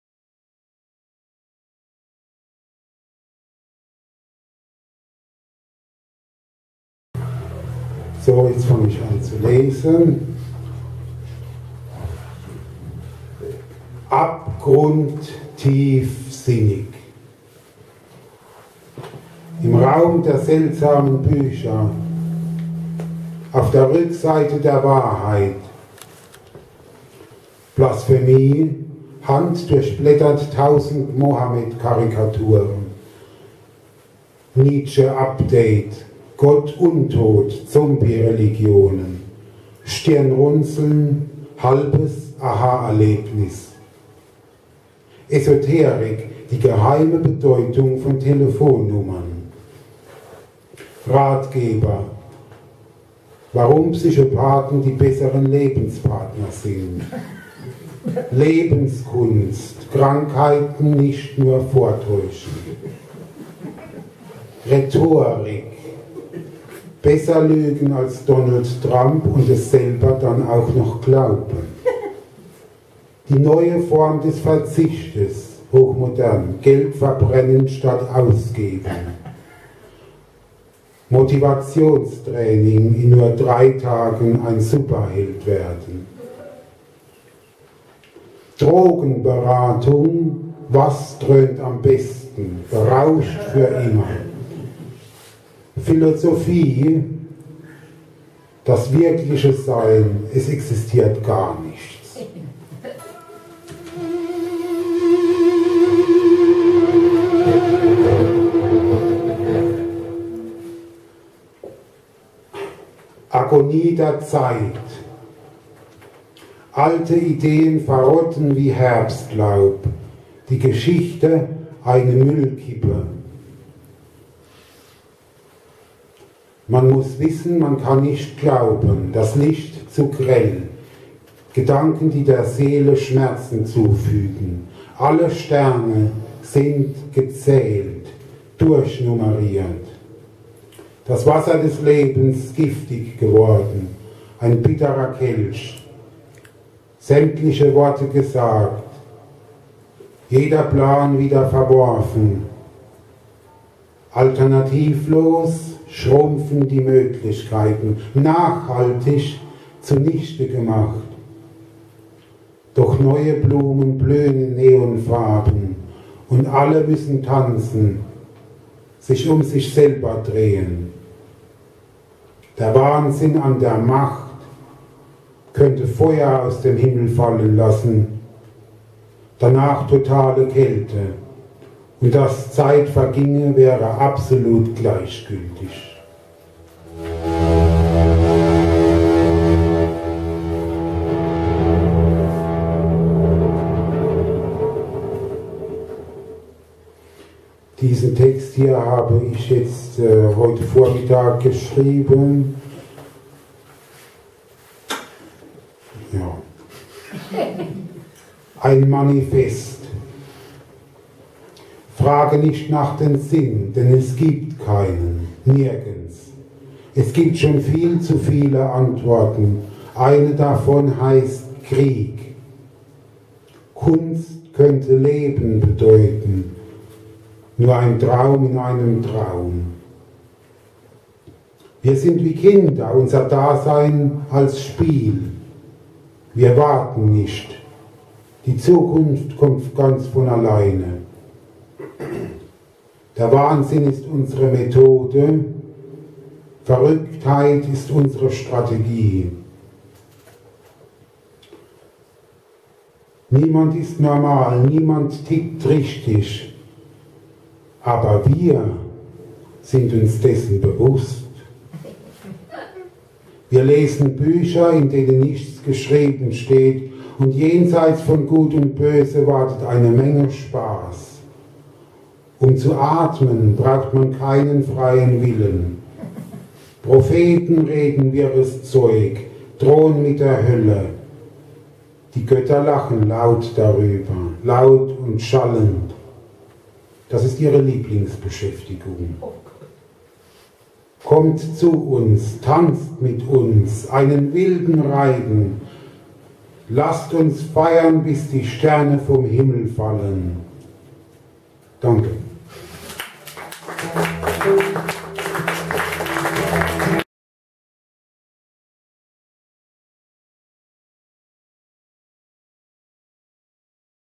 Euch blüht was Guido Lill
Frühlingslesung der Autorengruppe "Dichterzusamen" zu dem Thema "Euch blüht was"
Es liest Guido Lill